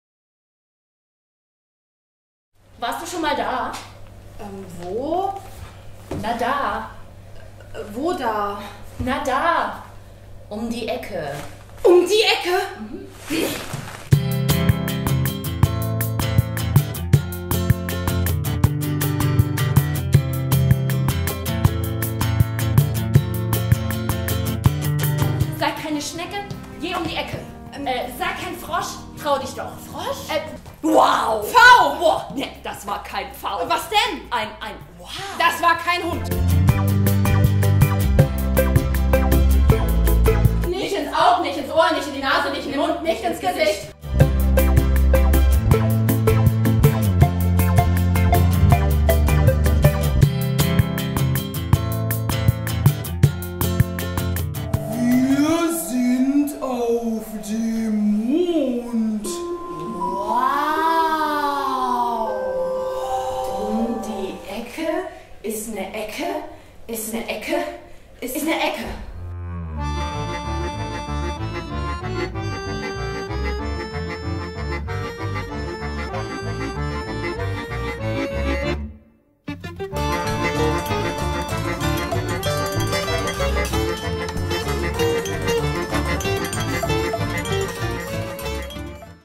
Theater Erlangen - UM DIE ECKE von Bernhard Studlar (ab 4)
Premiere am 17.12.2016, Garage

Hinter der nächsten Ecke kann man viel Neues und Überraschendes entdecken. Manchmal lauert dort aber auch Furchterregendes. Am besten ist es auf jeden Fall immer, zusammen auf Entdeckungsreise zu gehen!

Regie: Eva Veiders; Bühne und Kostüme: Anike Sedello; Dramaturgie: Jutta Körner
Mit Anika Herbst und Janina Zschernig